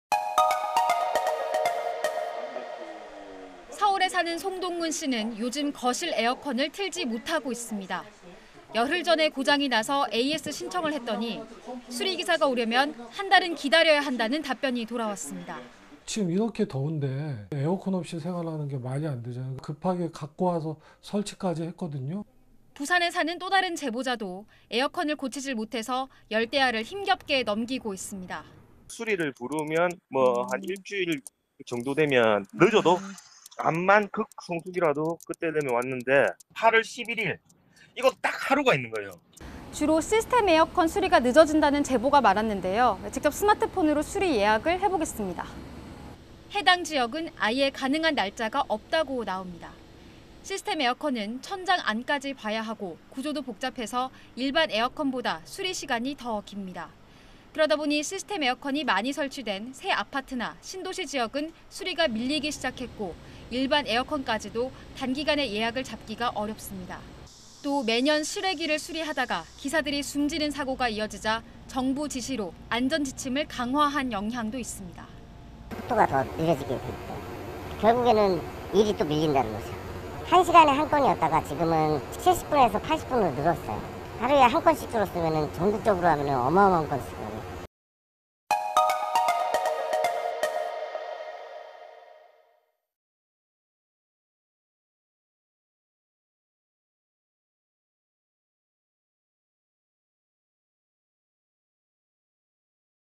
폭염 속 밀려드는 '수리'…지금 예약하면 '한 달'? (자막뉴스) / SBS
본격적인 여름으로 접어들었습니다.
장마철이라 습도까지 높은데요.
이렇게 덥고 습한 때 에어컨이 고장 나면 큰 불편을 겪는데 실제로 최근 에어컨 수리 요청을 하면 한 달 넘게 기다려야 하는 경우가 적지 않습니다.